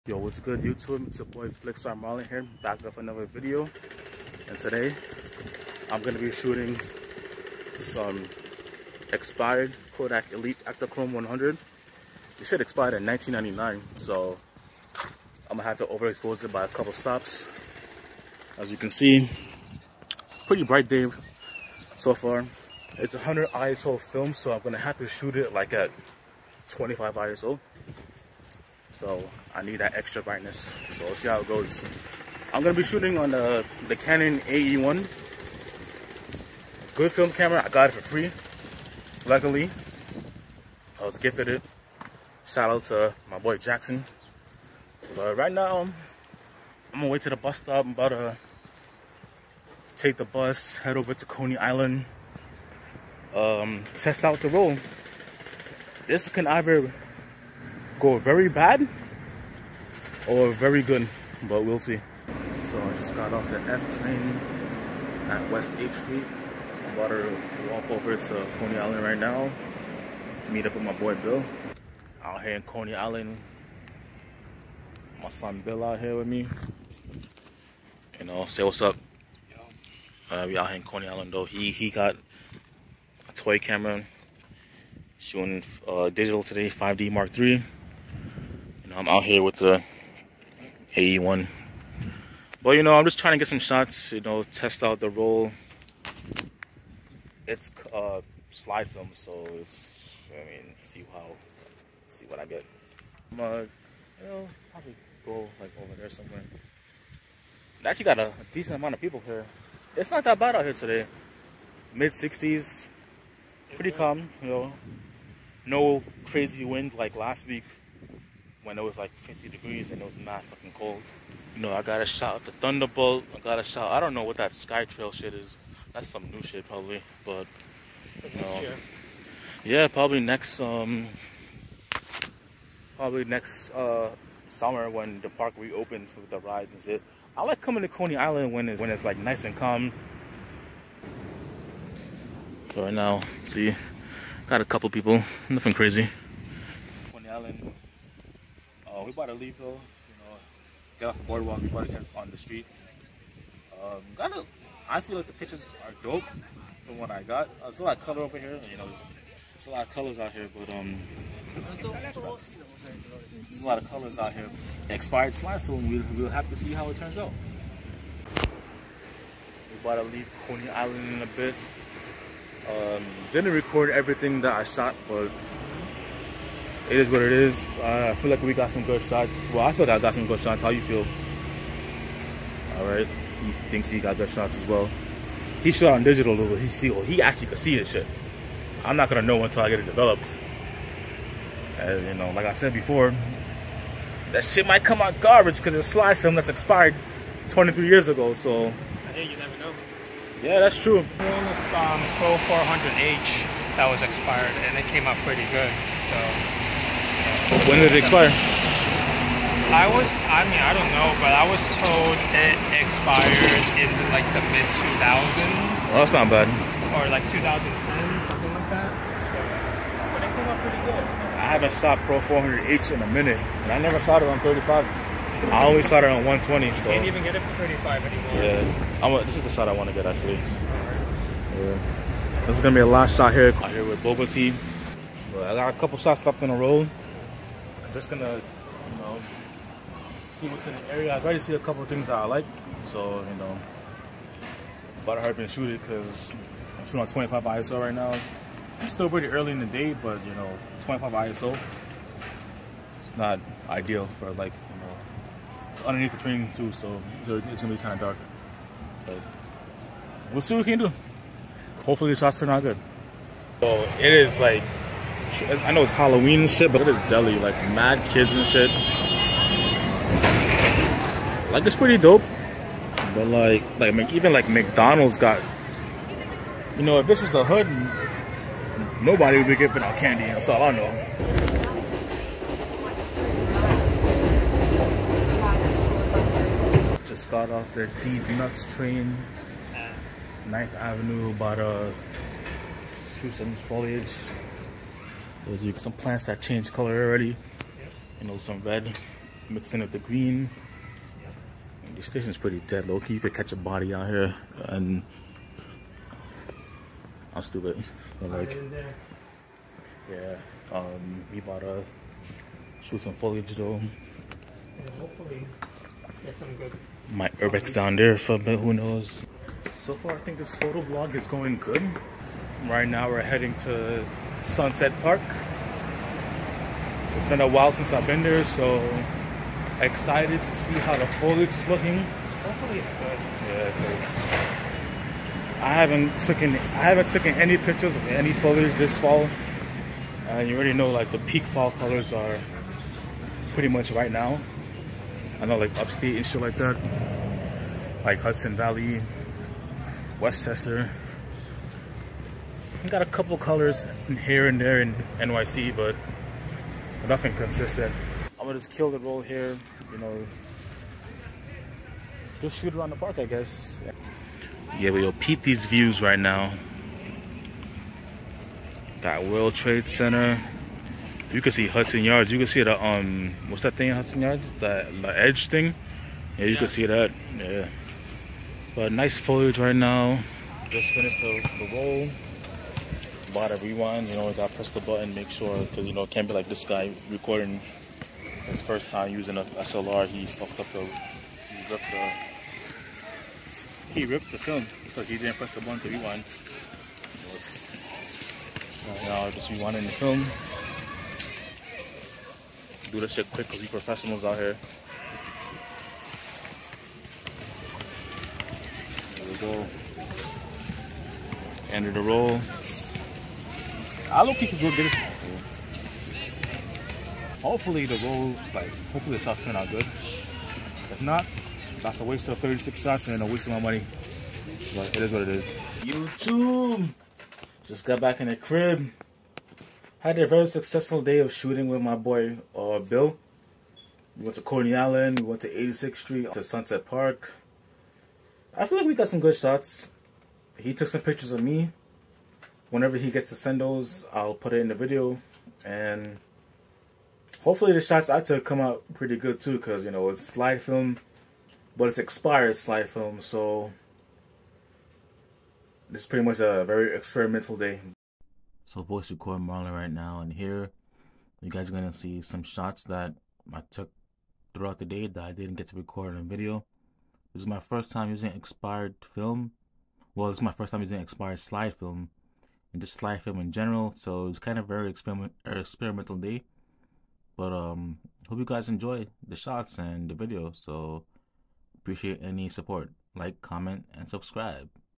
Shooting Expired Kodak Ektachrome Elite II 100 on Canon AE-1| Photography Vlog 005
This was my first time shooting slide film and it happened to be on a roll that EXPIRED 23 YEARS AGO!! This was a very experimental day and honestly didn't know what to expect with the results. Honestly, I'm pleased with what I got and I definitely need to try slide film again soon.